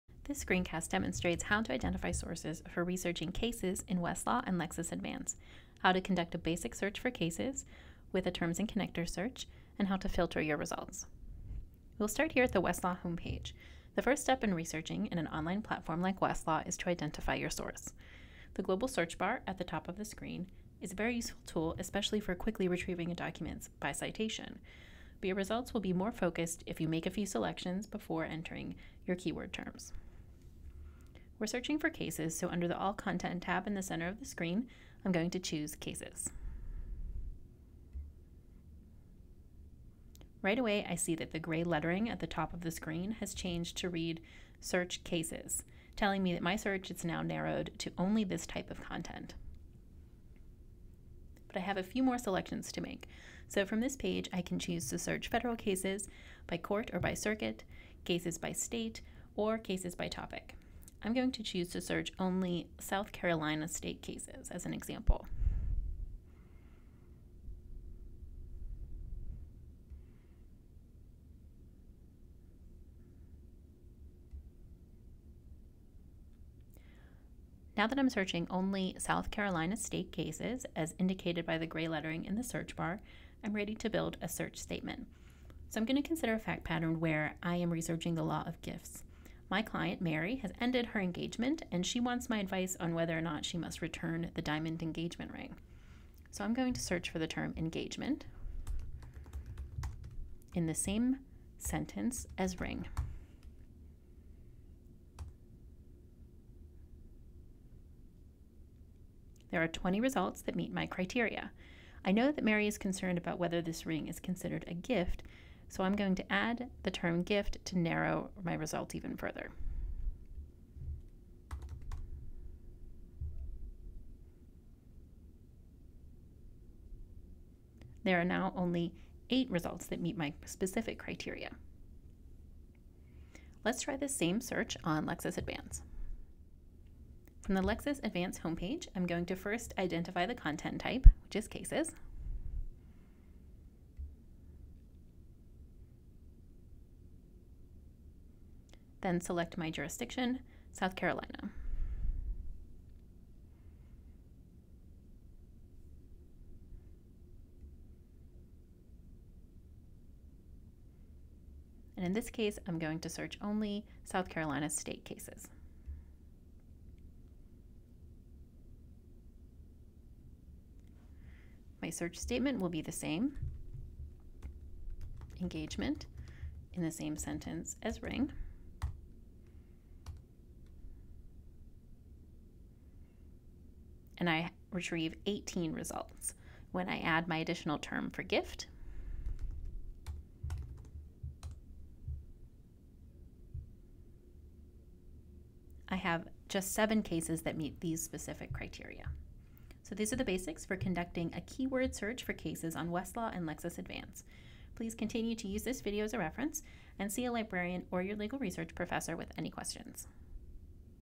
Searching for Cases on Westlaw and Lexis Advance 2018
This video demonstrates basic keyword searching on Westlaw and Lexis Advance